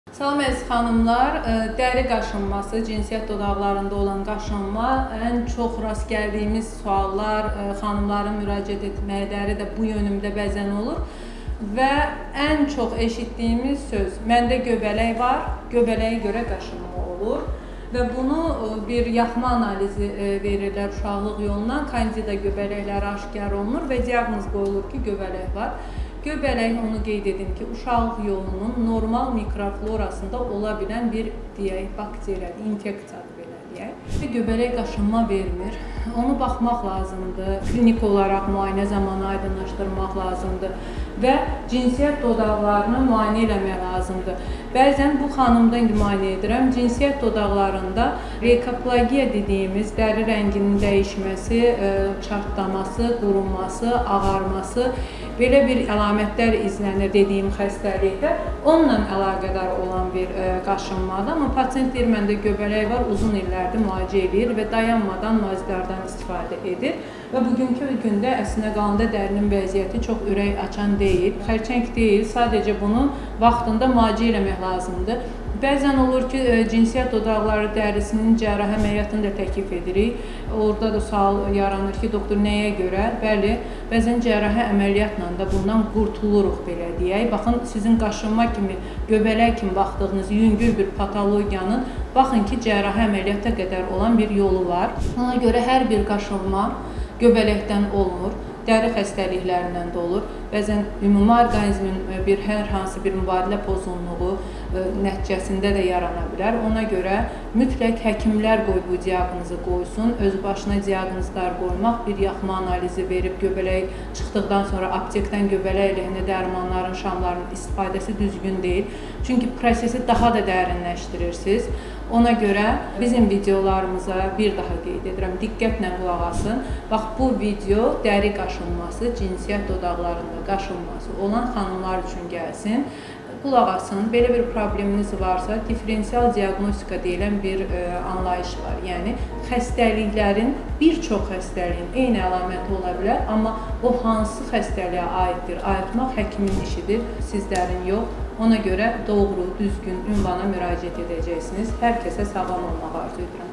QADIN CİNSİYYƏT ORQANINDA OLAN QAŞINMALARIN SƏBƏBİ NƏDİR❓BU QAŞINMALAR ƏMƏLİYYATA GƏTİRİB ÇIXARARMI❓
Bugün sizlərə cinsiyyət dodaqlarında olan qaşınmalar haqqında danışacağam. Bütün qaşınmalar göbələk sayılırmı❓Bu qaşınmalar əməliyyata gətirib çıxara bilərmi❓ Daha ətraflı videoda😊

📍 Milli Onkologiya Mərkəzi, Diaqnostika korpusu